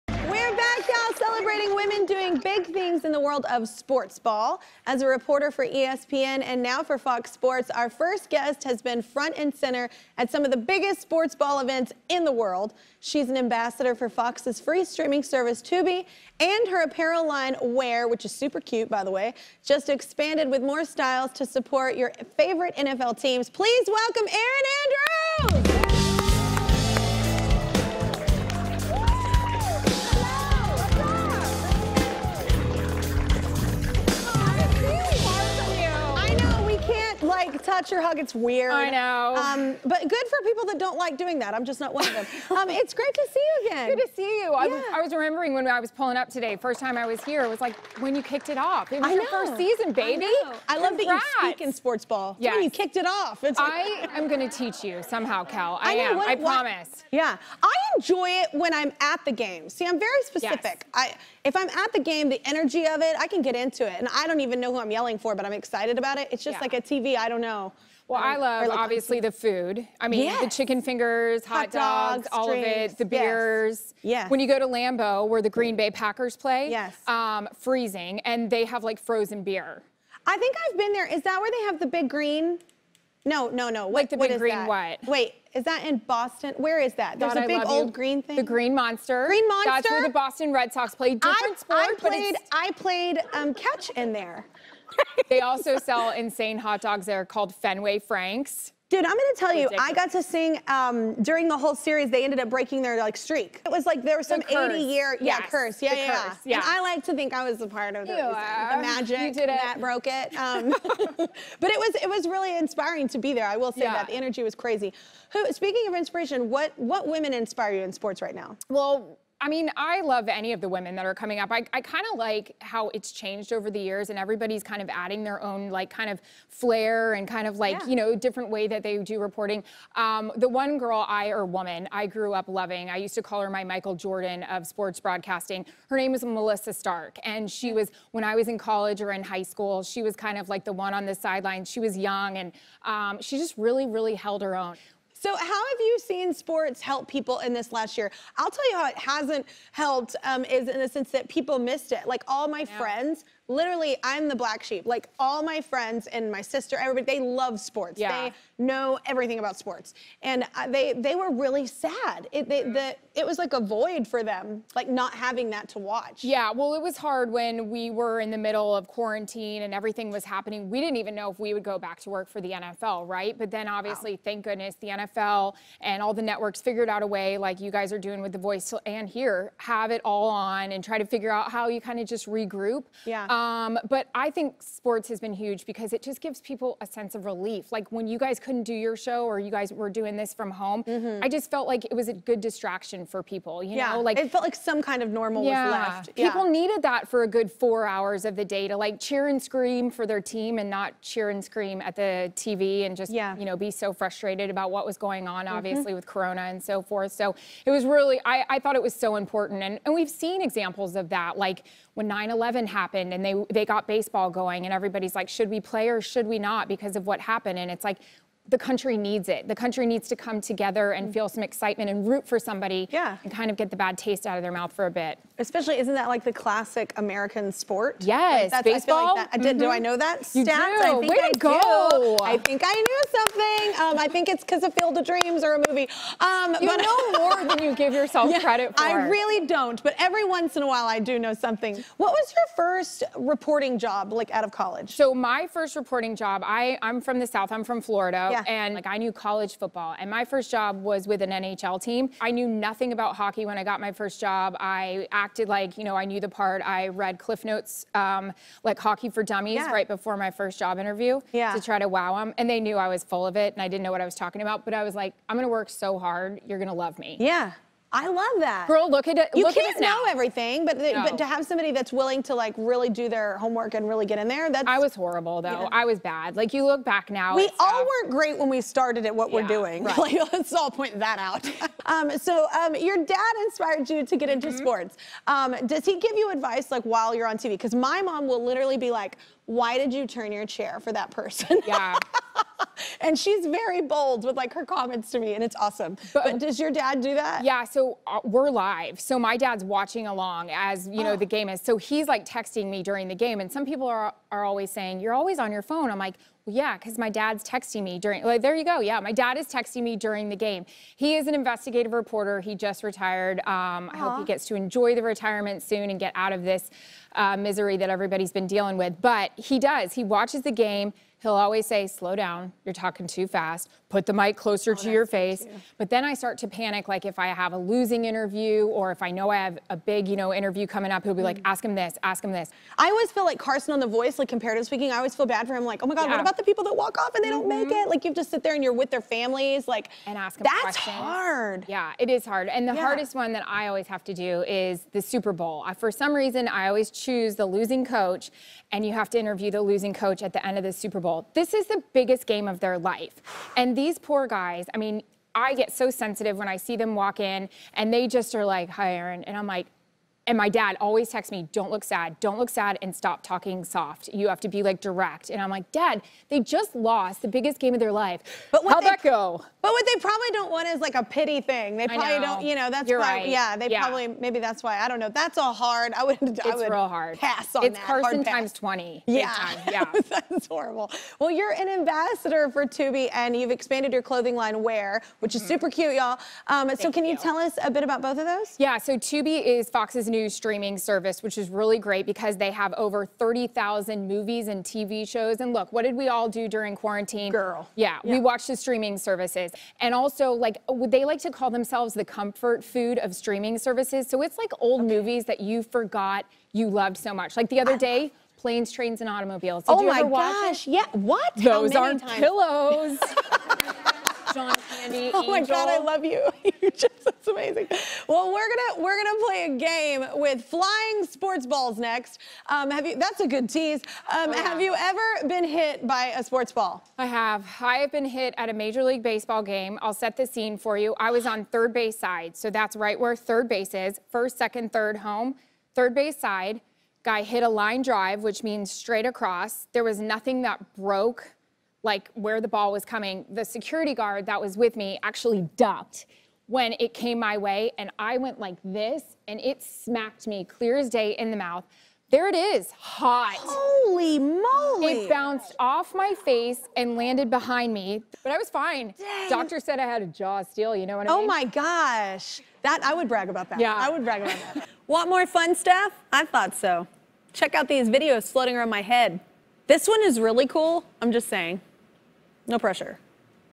Erin Andrews 'Panics' Before Interviewing Losing Super Bowl Teams
In this Ladies of Sportsball-themed show, Erin Andrews stops by to talk about who inspired her to become a broadcast journalist and admits that she "panics" when talking to losing coaches at the Super Bowl. Her dad tells her to be direct and not sad, but she can't help but feel bad when conducting those post-game interviews.